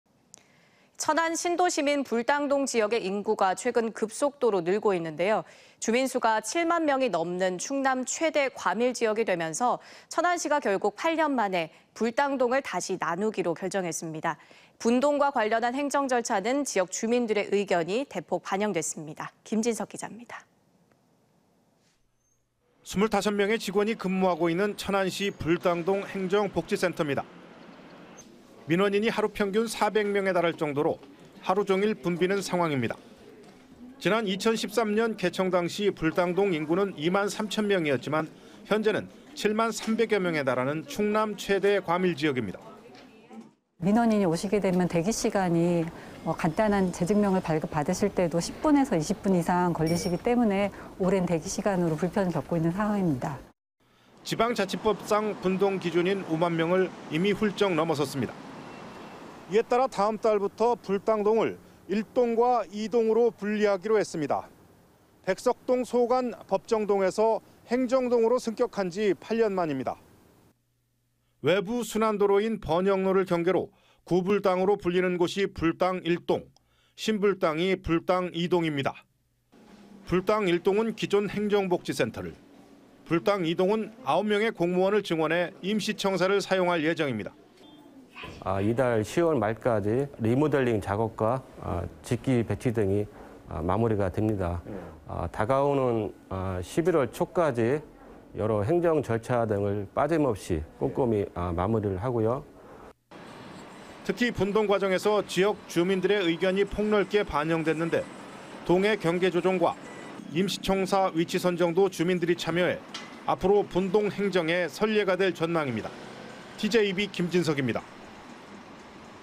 충남 최대 과밀지역..천안 불당동 11월 분동｜ TJB 대전·충남·세종뉴스
【 앵커멘트 】

천안 신도심인
불당동 지역의 인구가
최근 급속도로
늘고 있는데요.

주민 수가
7만 명이 넘는
충남 최대 과밀지역이 되면서
천안시가 결국 8년 만에
불당동을 다시 나누기로
결정했습니다.

분동과 관련한 행정절차는
지역 주민들의 의견이 대폭
반영됐습니다.

보도에 김진석 기자입니다.